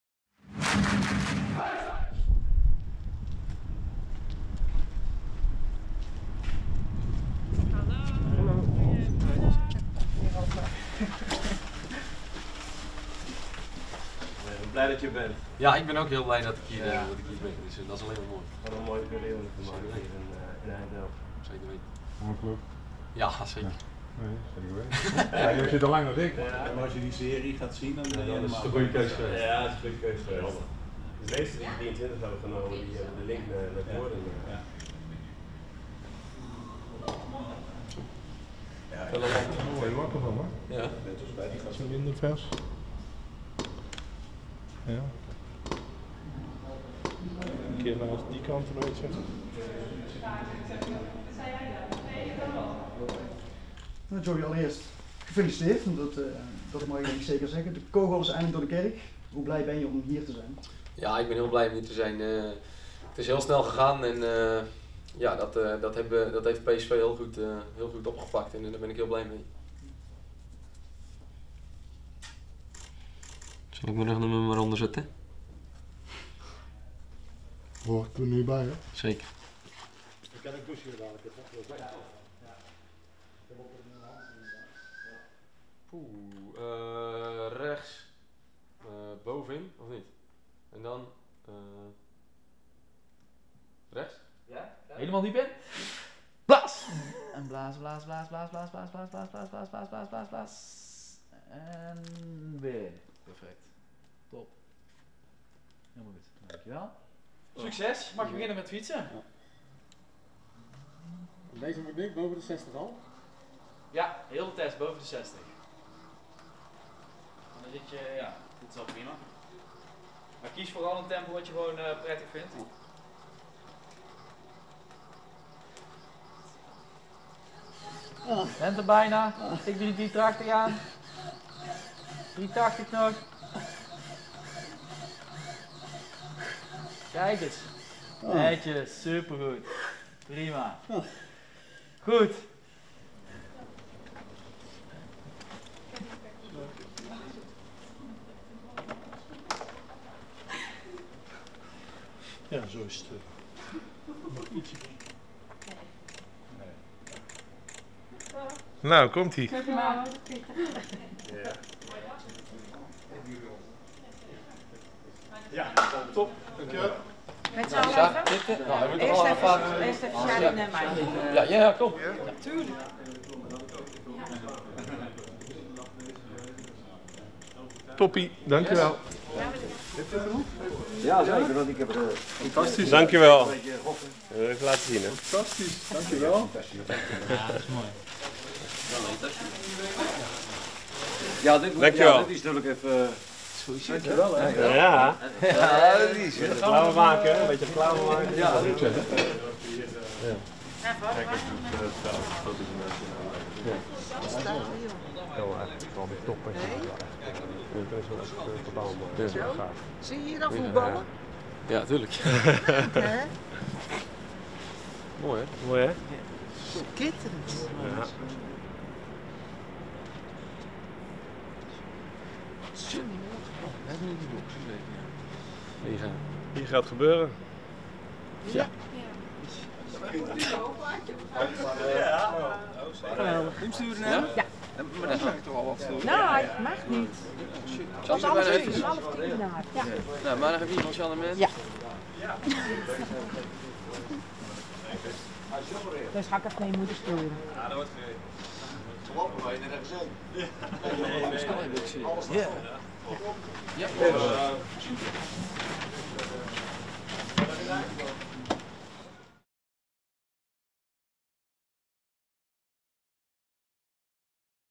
VEERMAN's first day in EINDHOVEN 📸📲⛑🖊 | INSIDE
Een unieke inkijk in de eerste dag van Joey Veerman als PSV'er. Wil jij het gesigneerde shirt winnen van Veerman?